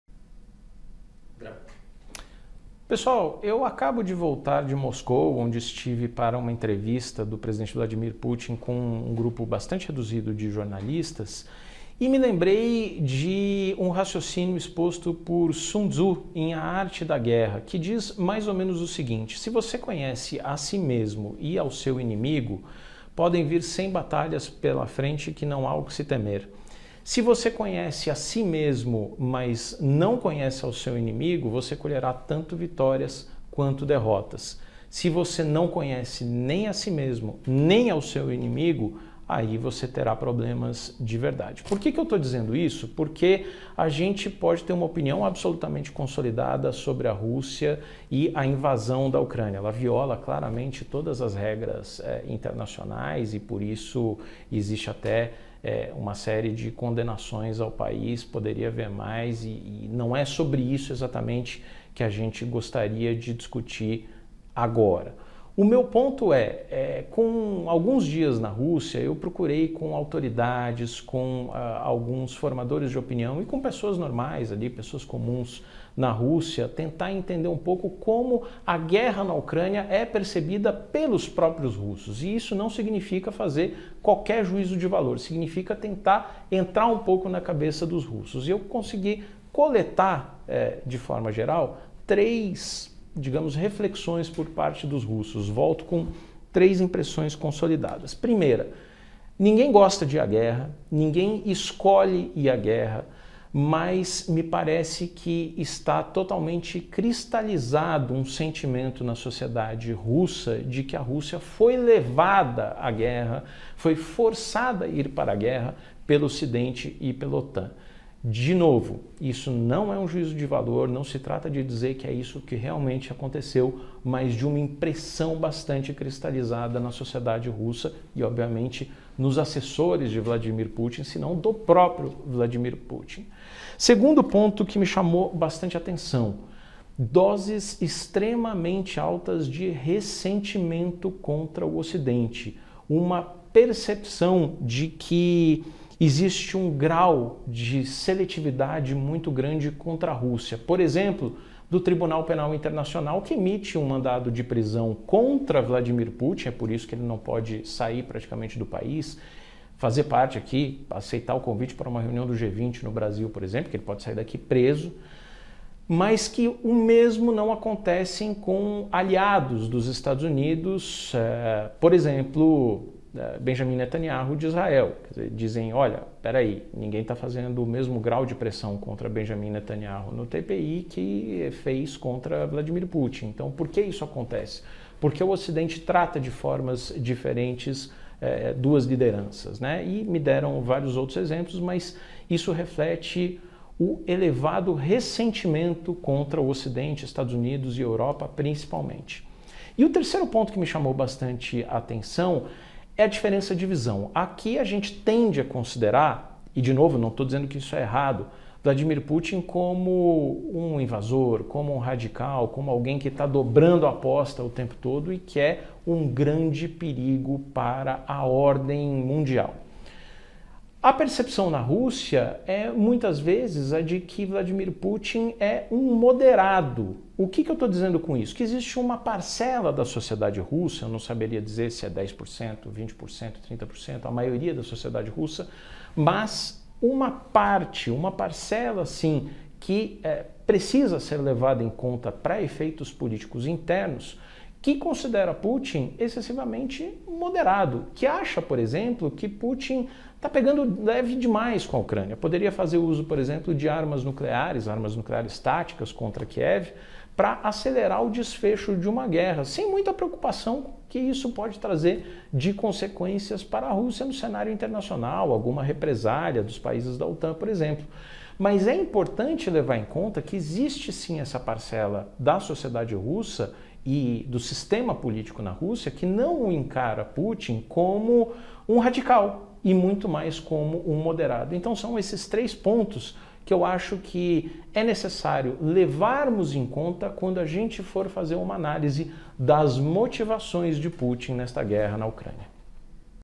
Daniel Rittner: Os motivos da Guerra na Ucrânia na visão dos russos
O diretor da CNN Daniel Rittner comenta como a guerra na Ucrânia é percebida pelas pessoas na Rússia e de que forma isso pode auxiliar na compreensão do conflito.